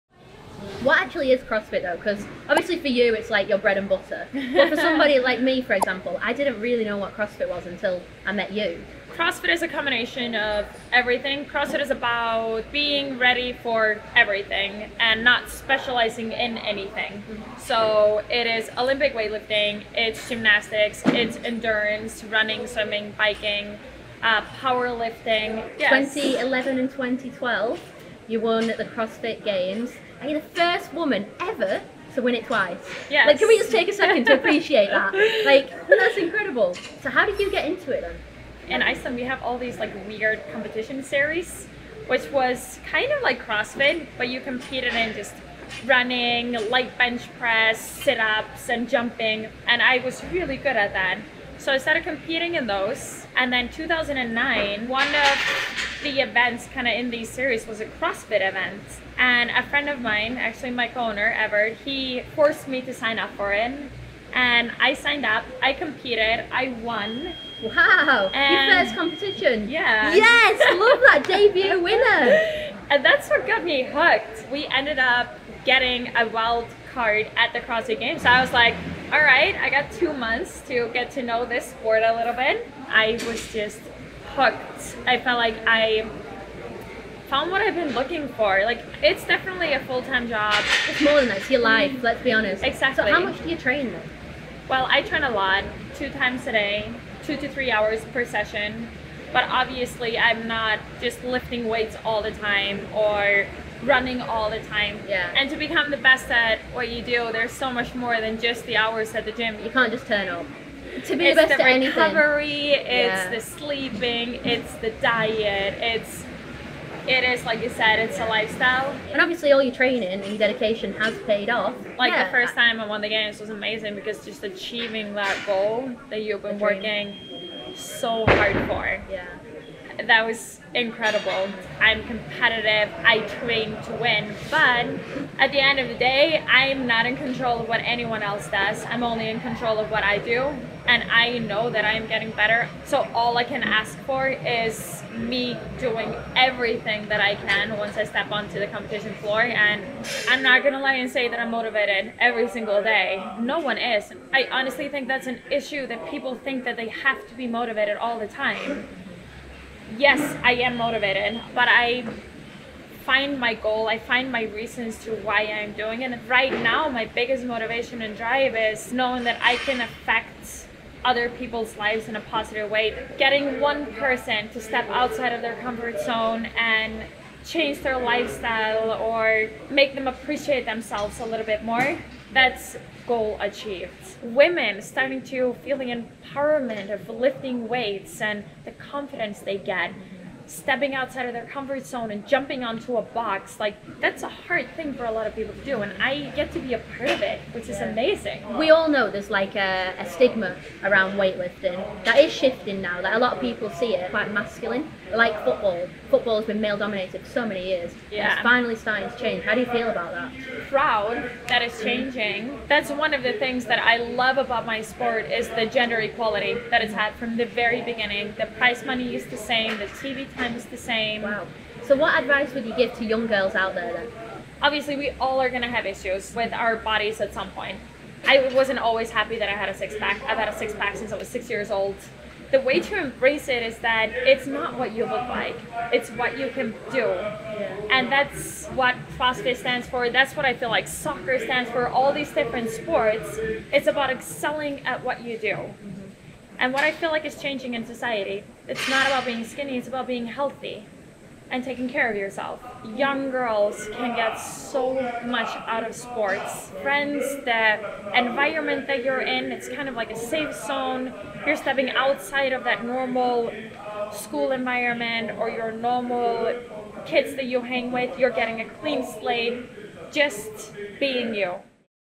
🏋️‍♂️ Training to WIN with 2x World's Fittest Woman, Annie Thorisdottir! PlayAnywhere 🇮🇸
Hey, Liv here 🙋

Welcome to an extended version of PlayAnywhere Reykvaík. During the shoot, I got to sit down with 2 x World's Fittest Woman, Annie Thorisdottir and we chatted all things cross fit, masculine stigmas and what it takes to become 2x World's Fittest Woman!

- What is cross fit?
- 2 x World's fittest Woman
- How did you get into cross fit?
- How much do you train?
- Masculine stigma for Crossfit and Football - discuss
- What advice would you give to young girls out there?